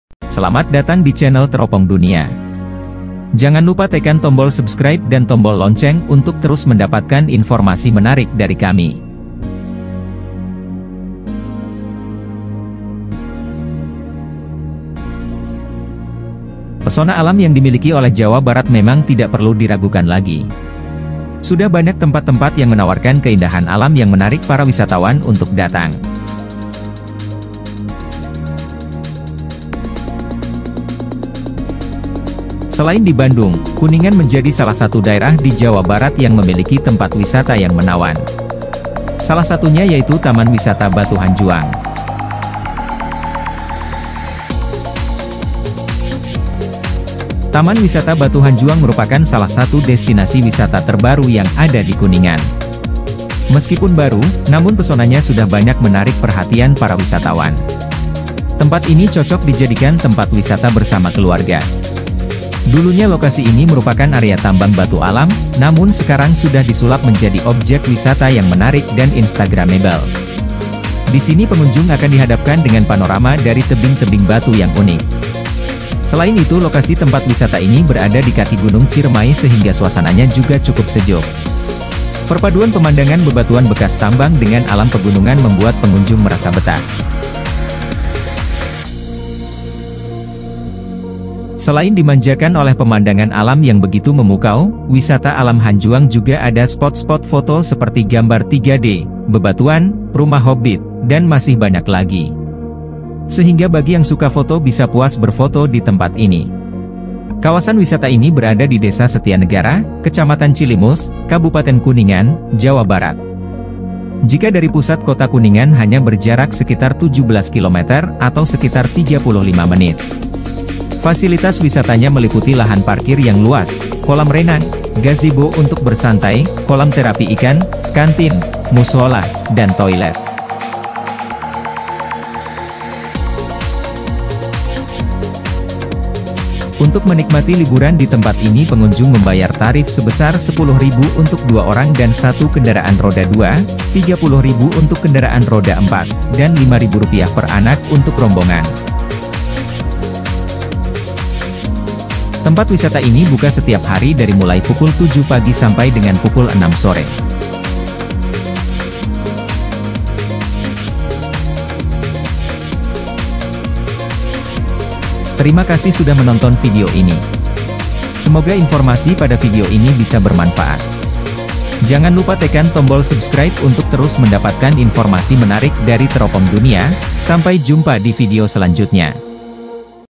Taman Wisata Batu Hanjuang Kuningan Jawa Barat Liburan Keluarga di Kolam Renang Taman Batu Hanjuang
Buat kamu yang bosan dengan susasana wisata taman yang itu-itu saja, Taman Batu ini cocok buat kamu masukan ke dalam list traveling kamu selanjutnya. Alih-alih menawarkan pemandangan pepohonan, wisata taman batu hanjuang menawarkan pemandangan susunan batu dan tebing-tebing alami yang tersusun rapi.